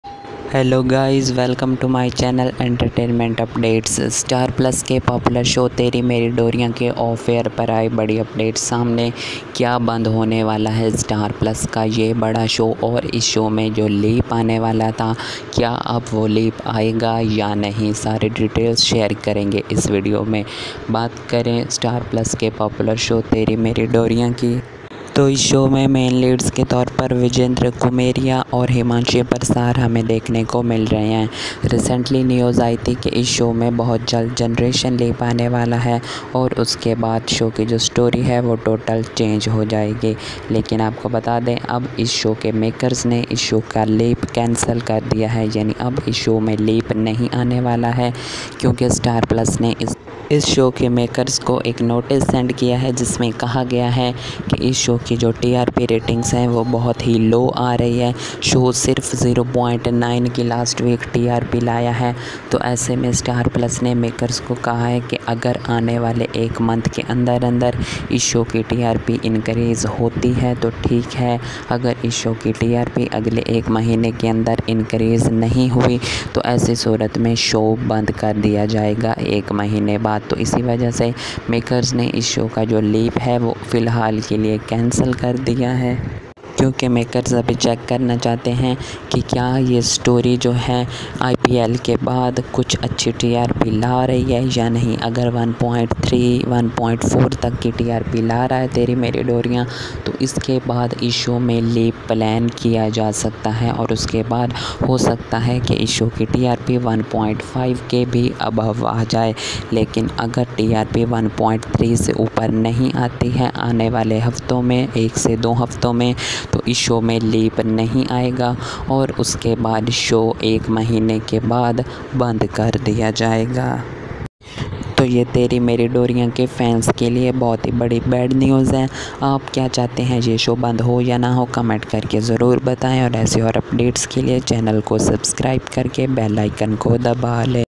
Teri Meri Doriyan to Go Off Air in One Month | Leap Cancel of Show | Star Plus sent notice to Serial
Teri meri doriyan going Off Air
Teri meri doriyan last episode kab ayega
Vijendra kumeria
Himanshi Parashar
Will Teri meri doriyan TRP increase